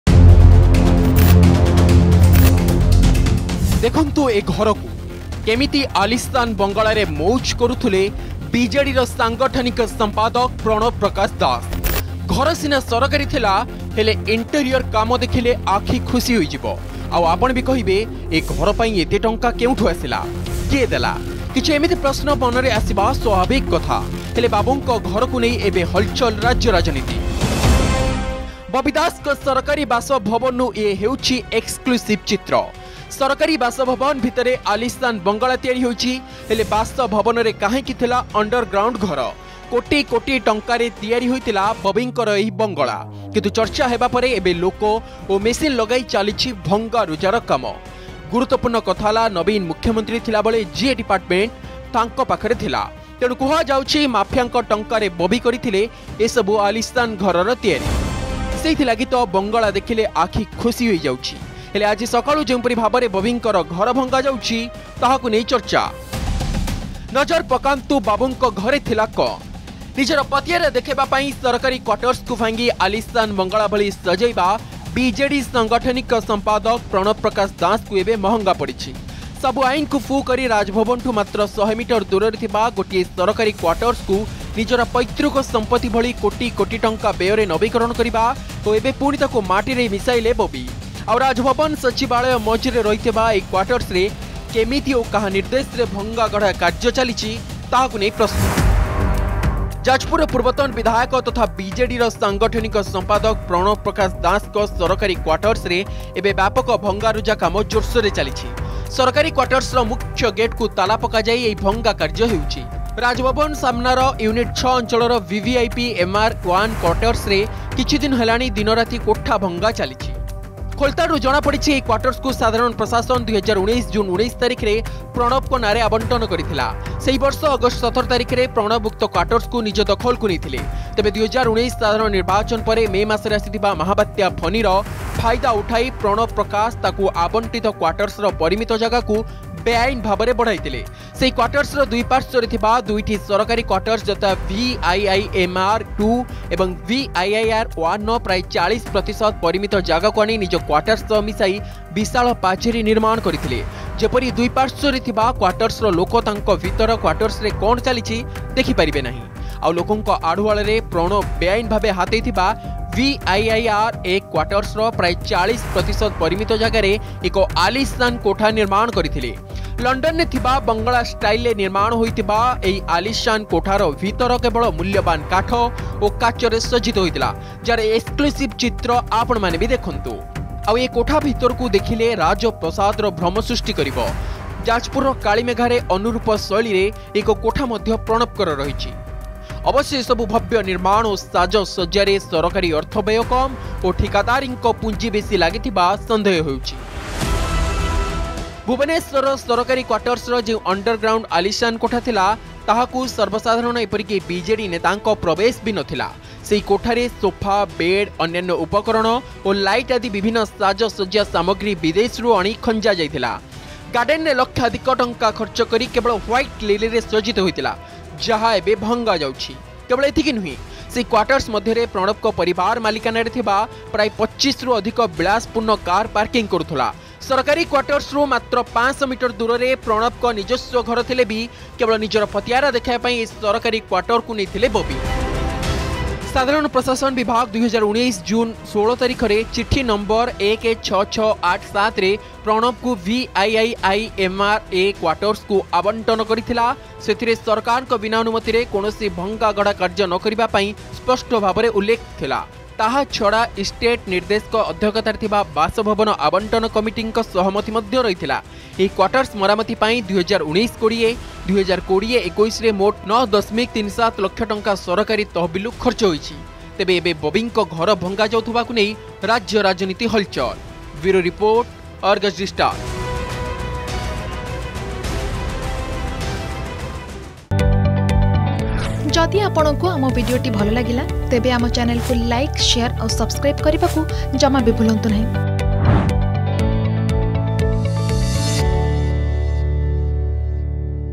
These Unique Facilities Found in BJD Leader Bobby Das's Underground Rooms of Government Quarter
ବବିଙ୍କ ଅଣ୍ଡରଗ୍ରାଉଣ୍ଡ ଘରୁ ମିଳିଲା ଏଇ ସବୁ ଜିନିଷ । ଜିଏ ବିଭାଗ ଟେରା, ଆଲିଶାନ ଘର ବାବଦକୁ ଖର୍ଚ୍ଚ ହୋଇଥିଲା